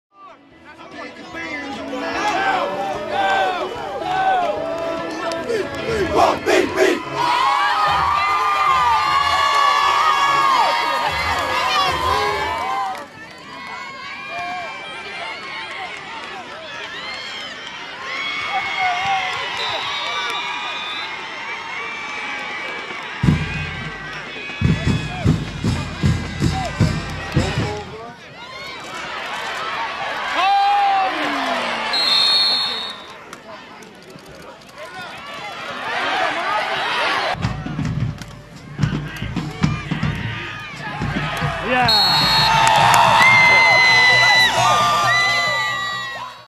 DH Conley plays EE Smith, Nov. 15
D.H. Conley faced E.E. Smith in the first round of playoffs on Nov. 15. Conley won 35-15. Conley will play J.H. Rose on Nov. 22.